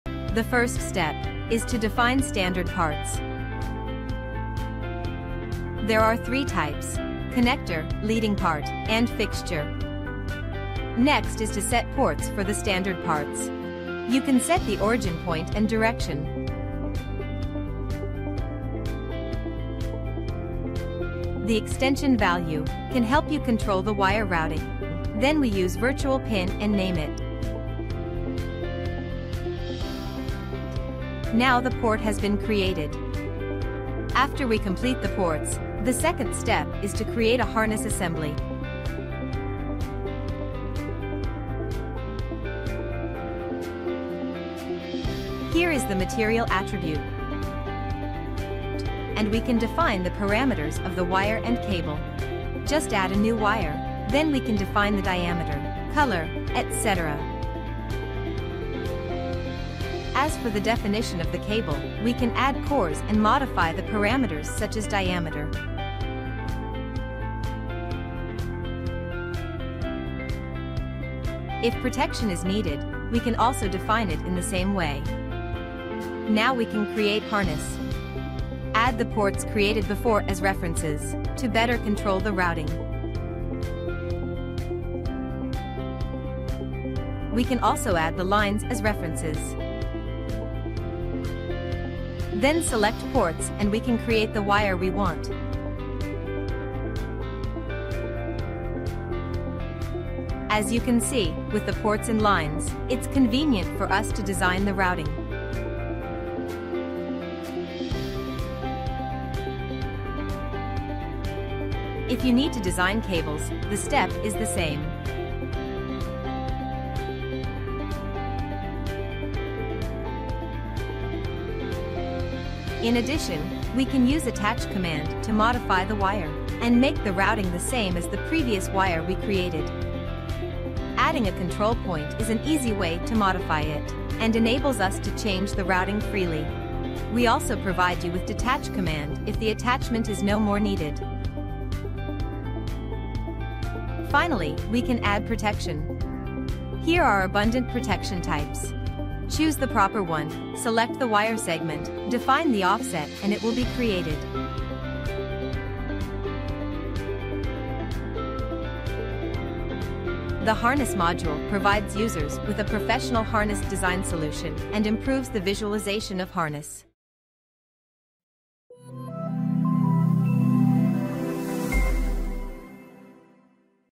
ZW3D - Harness Design
Harness Module sudah ada pada ZW3D 2024 tipe Advanced, modul ini memudahkan engineer untuk mendesain harness.

Hubungi kami untuk informasi lebih lanjut.